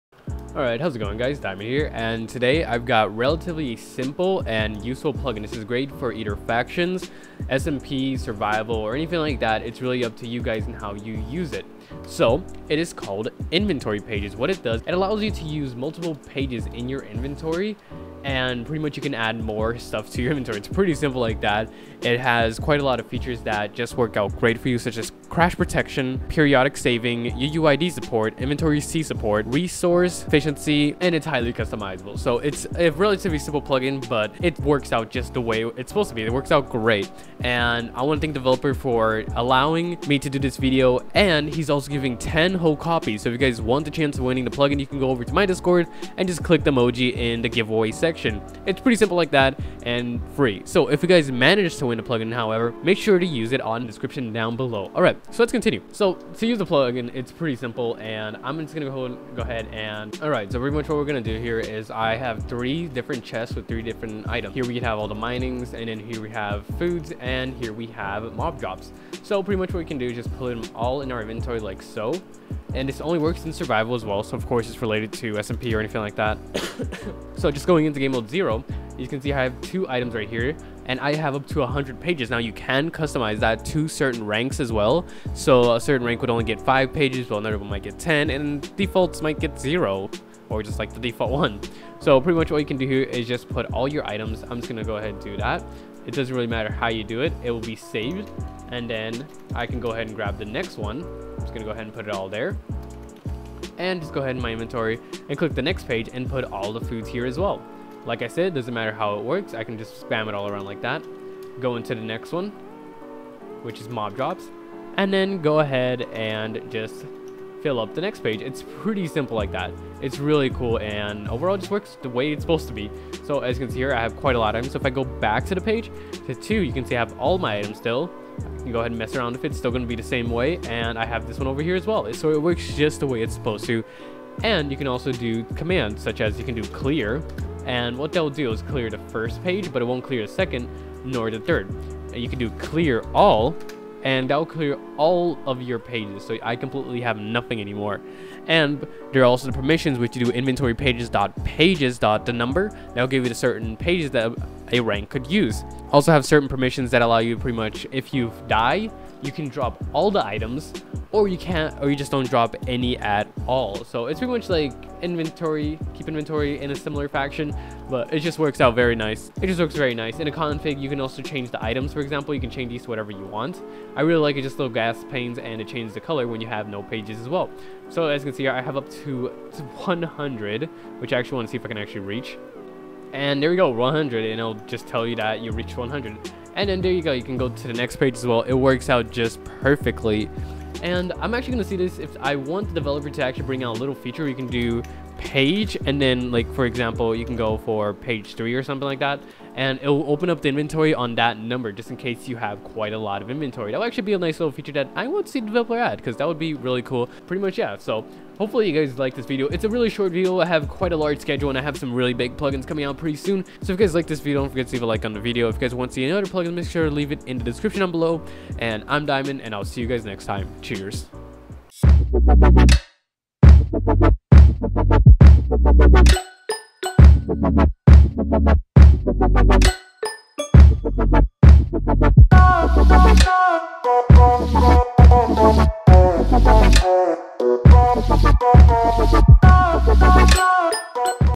InventoryPages Plugin
●Hey DiamondRushXD here, InventoryPages is simple it can add pages to your inventory for extra storage!